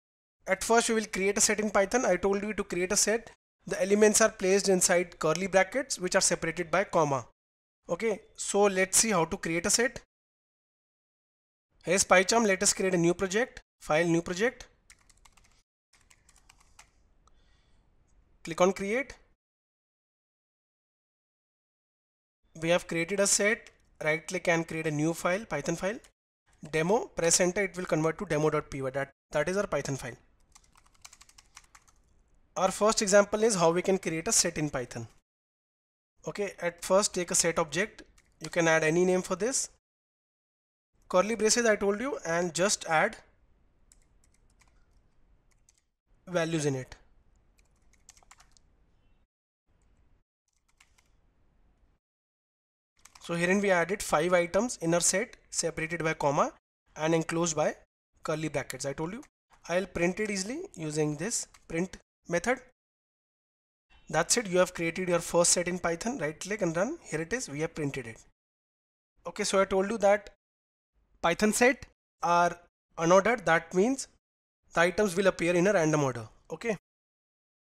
How to create a Set in Python | Tutorial for Beginners
In this video, learn how to create a Set in Python. A Set is a collection in Python. To create a set, place the elements inside curly brackets, separated by comma.